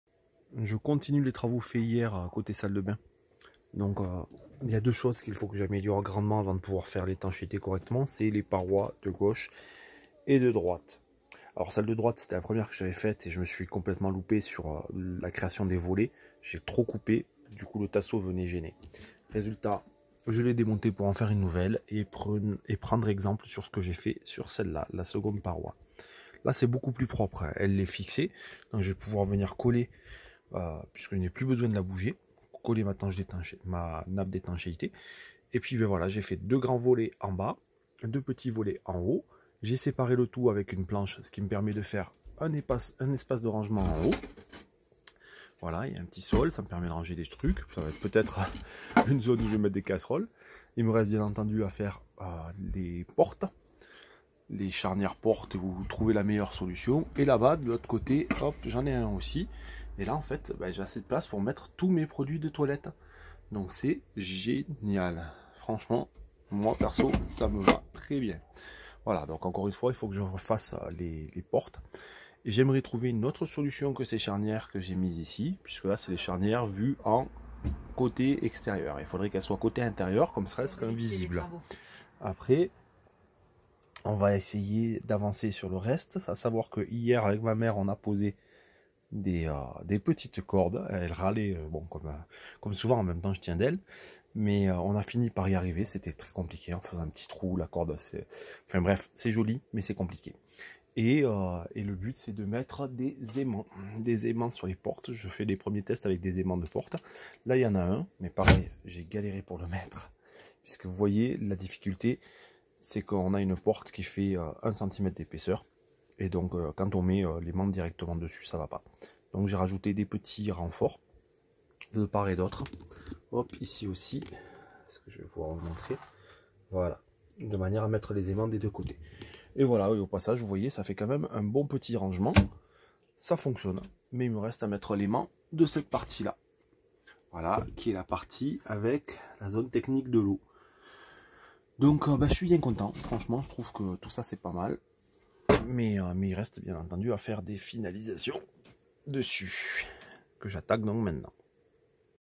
BREVE - Salle de bain : Suite des travaux sur le meuble gauche
Voilà la suite des travaux sur le meuble gauche, sous le chauffe-eau :)

IMPORTANT
Il s'agit d'une vidéo de la série "Brèves et brutes", au format court et sans réel montage.
Elle fait partie d'une série qui te permet de suivre l'aménagement de mon véhicule

Retrouver les brèves au format story sur mon compte instagram ou plus d'infos sur mon site web